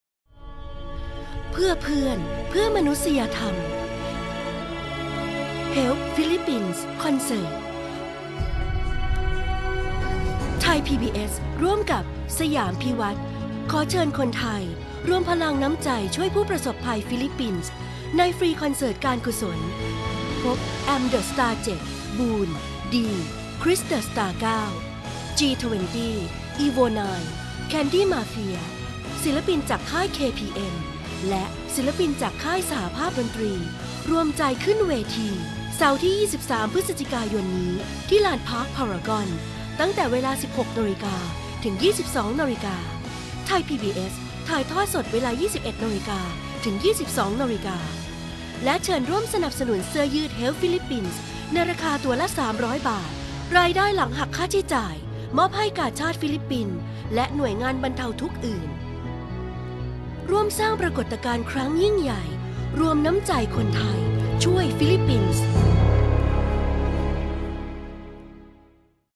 คอนเสิร์ตการกุศล Help Philippines
For friend For Humanity เพื่อ..เพื่อน เพื่อ..มนุษยธรรม

ThaiPBS ร่วมกับ SIAM PIWAT ขอเชิญคนไทยรวมพลังน้ำใจช่วยผู้ประสบภัยฟิลิปปินส์ ในฟรีคอนเสิร์ตการกุศล...พบ แอมป์ The Star 7, บูรณ์ / ดี / คริส The star 9, G20 / Evo Nine / Candy Mafia /ศิลปินจาก KPN และศิลปินจากสหภาพดนตรีรวมใจขึ้นเวที
...เสาร์ที่ 23 พ.ย.นี้ ที่ลานพาร์ค พารากอน ตั้งแต่เวลา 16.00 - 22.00 น.
ไทยพีบีเอส ถ่ายทอดสดในเวลา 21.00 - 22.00 น.
...และเชิญร่วมสนับสนุนเสื้อยืด Help Philippines ราคาตัวละ 300 บาท
รายได้หลังหักค่าใช้จ่ายมอบให้ กาชาดฟิลิปปินส์ และหน่วยงานบรรเทาทุกข์อื่น
...ภายในงานชวนร่วมกิจกรรมดีๆ "Post & Share to Help Philippines" ง่ายๆ เพียงโพสต์รูปถ่ายในงาน อัพโหลด ‪#‎Instagram‬ ติด tag ‪#‎HelpPhilippines‬ แล้วรอรับรูปผ่าน Instaprint เป็นที่ระลึกได้เลยค่ะ

"ร่วมสร้างปรากฏการณ์ครั้งยิ่งใหญ่ รวมน้ำใจคนไทยช่วยฟิลิปปินส์"